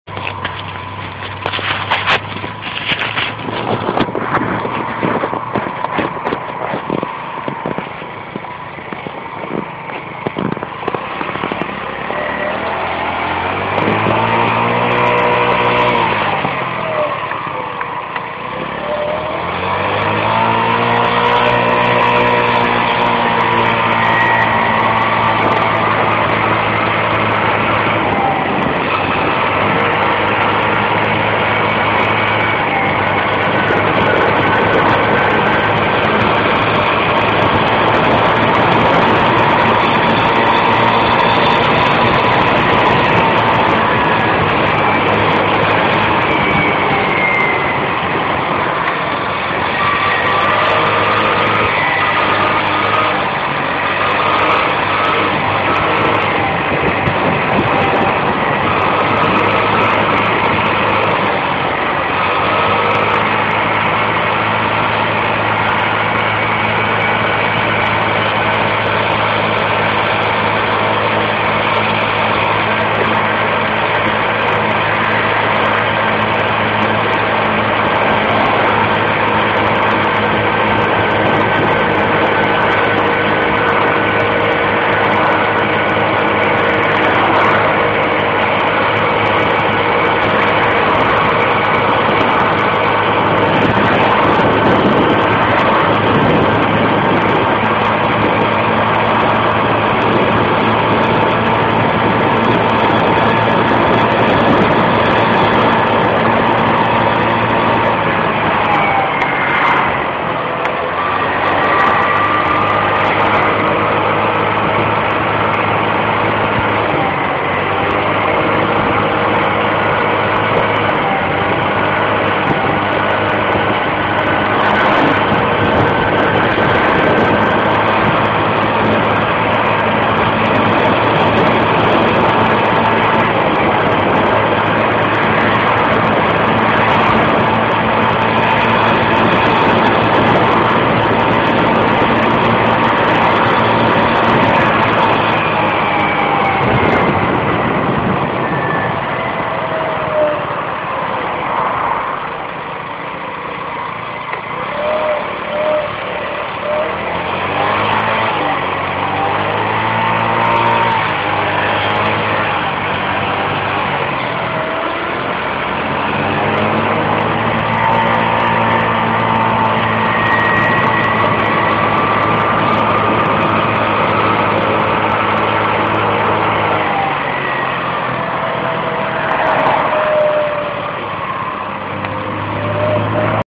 Jazda po Sokołowie
Spokojna jazda skuterem przez pół miasta.